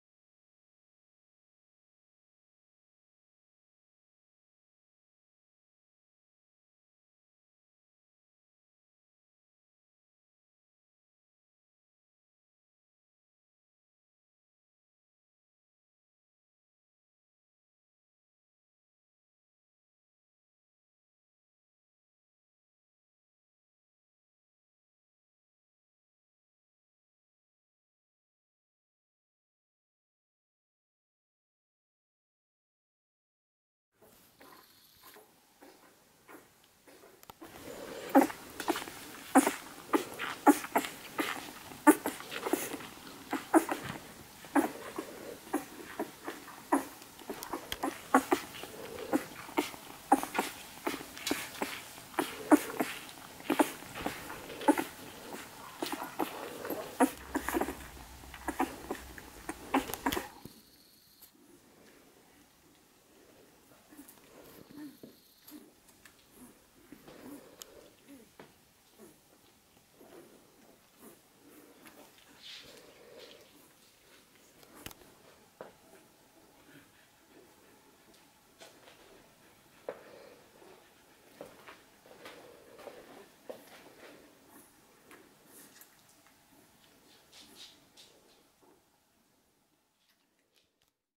COLLIE rough puppies, milking
COLLIE rough puppies (video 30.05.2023)
Strong and powerful puppies, they even made big hickeys on their mother's belly :-P Little leeches ;-)
When they drink milk, you can even hear it flowing!

sire: JunCh.PL, Ch.MERIDEON Actis PL
dam: JunCh.PL TERRAVITA Actis PL